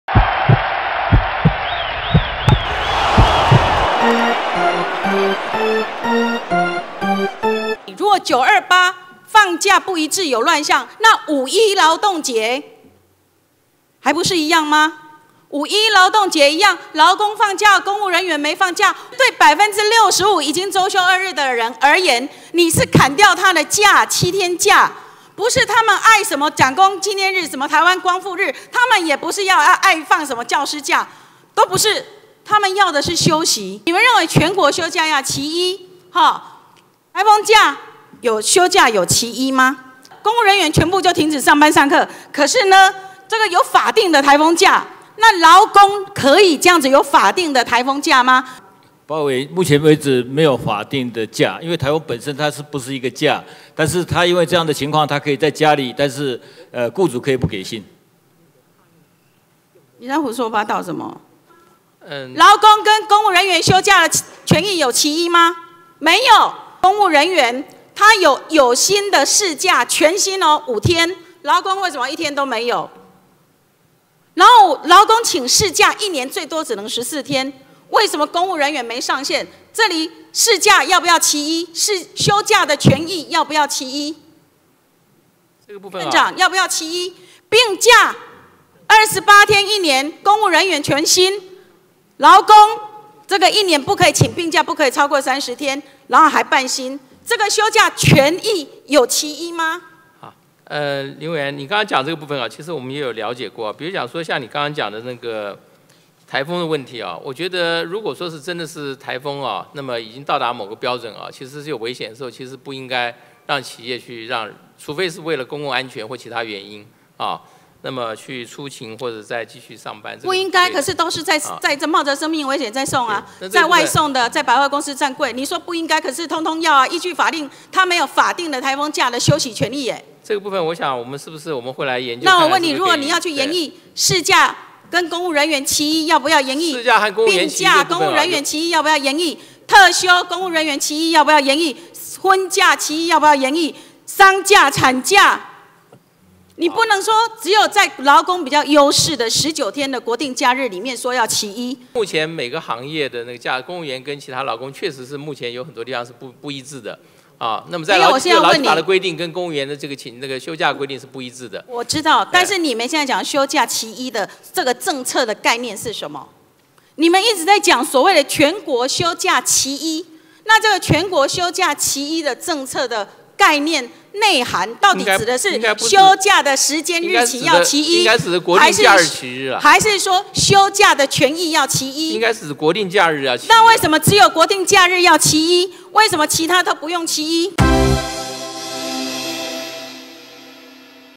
【沃草好球】林淑芬：政府喊「國定假日齊一」 颱風假怎不讓勞工和公務員齊一？
沃草／影音：陳東；文字：薛翰駿

「七天國定假日」成為新政府的燙手山芋，民進黨立委林淑芬今年四月擔任召委時把馬英九政府刪除的七天國定假日恢復，但蔡英文政府上台後打算在推動《勞基法》一例一休修法後，再次從《勞基法施行細則》中刪除七天國定假日。

林淑芬9月20日在院會的施政總質詢質疑，勞工的《勞基法施行細則》中國定假日數目的確比公務員的《公務人員週休二日實施辦法》中的國定假日多出七天，但是公務員的事假、病假、特休假規定明顯都優於勞工許多，甚至連颱風假都是只有公務員才享有的，政府說要「休假齊一」砍七天假，怎麼不先把勞工不如公務員的放假規定齊一？

林淑芬質疑，勞工是不是有沒有法定的颱風假？勞動部長郭芳煜回答，勞工確實沒有法定的颱風假，但若政府宣佈當天放颱風假，勞工可以選擇不來上班，雇主則可以不發當天的薪水。林淑芬聽了之後痛罵郭芳煜「胡說八道」，不懂民間實際運作的狀況。她指出，像是外送人員、百貨公司櫃姐都會被雇主強迫上班，照樣出勤，連人命都不等值嗎？

林淑芬也質疑，公務員每年可以請五天有薪事假，勞工一天都沒有，而扣薪水的事假公務員可以無限請，勞工卻只能請十四天。公務員還享有領全薪的病假二十八天，勞工能請的三十天病假卻只能領一半薪水。更不用提差異極大的特休假規定，質疑林全怎麼不把這些假的規定先「齊一」？林全則回應勞工和公務員的休假規定確實有差異，日後會研議改進，並解釋政府說的「齊一」只有國定假日。